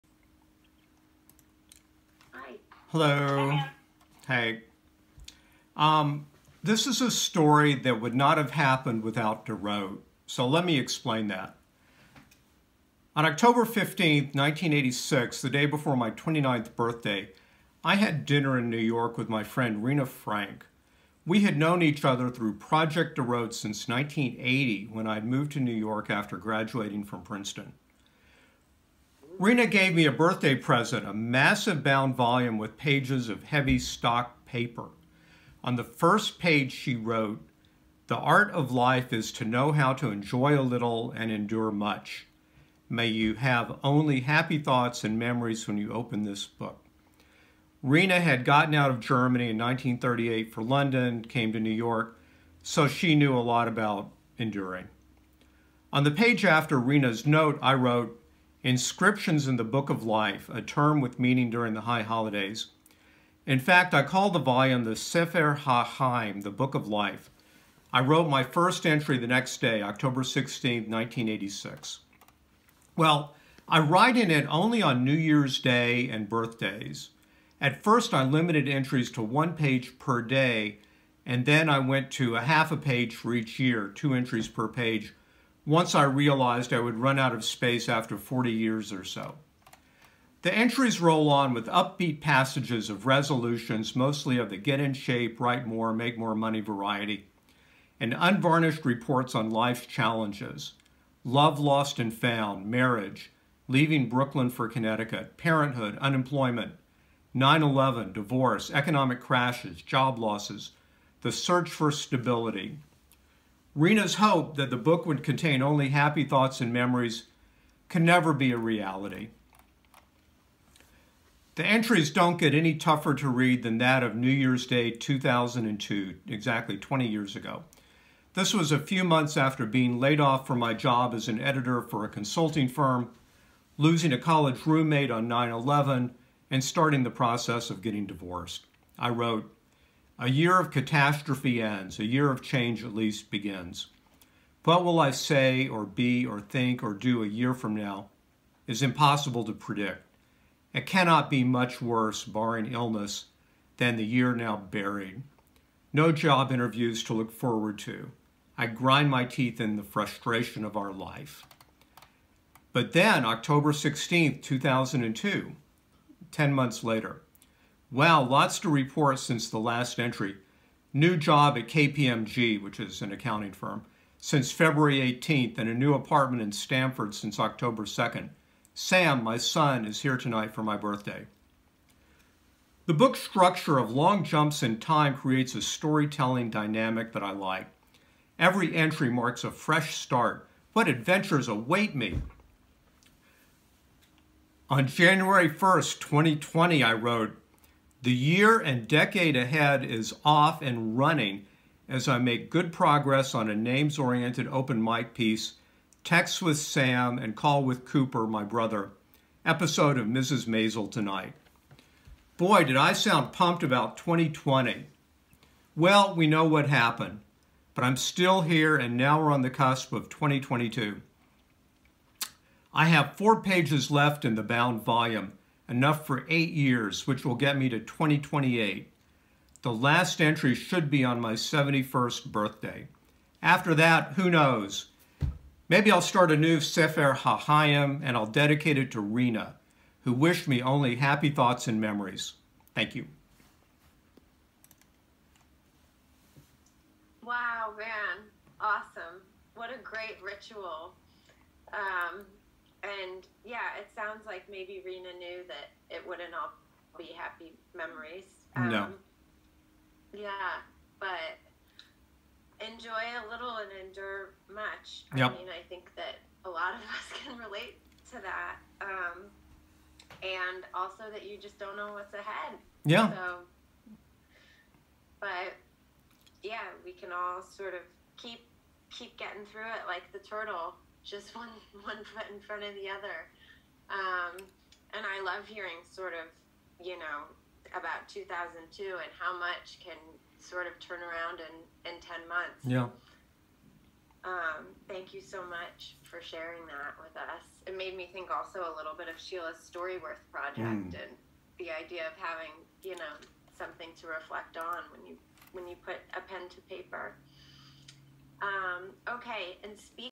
Ready for Another Entry in Rena’s Book of Life
Thanks to a gift of a bound volume from a friend, I have been recording my thoughts on New Year’s Day and my birthday since 1986, come what may. The entries give a twice-yearly recap of what’s going on in my life and the world. I was so pumped going into 2020 . . .